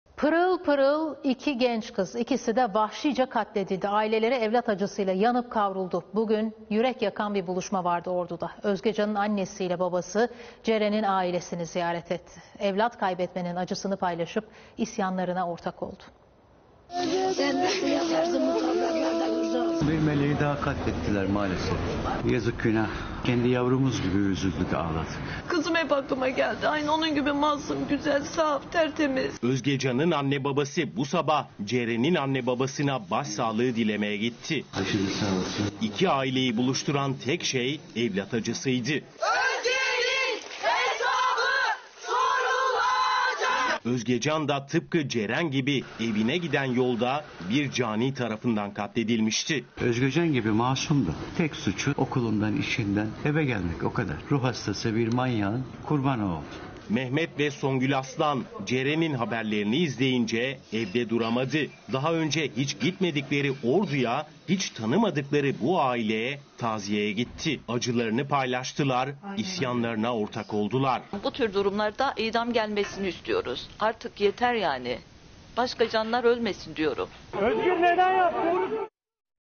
Özgecan'ın ailesi, Ceren'in ailesini ziyaret etti
PIRIL PIRIL İKİ GENÇ KIZ... İKİSİ DE VAHŞİCE KATLEDİLDİ... AİLELERİ EVLAT ACISIYLA YANIP KAVRULDU... BUGÜN YÜREK YAKAN BİR BULUŞMA VARDI ORDU'DA... ÖZGECAN'IN ANNESİ İLE BABASI, CEREN'İN AİLESİNİ ZİYARET ETTİ... EVLAT KAYBETMENİN ACISINI PAYLAŞIP, İSYANLARINA ORTAK OLDULAR...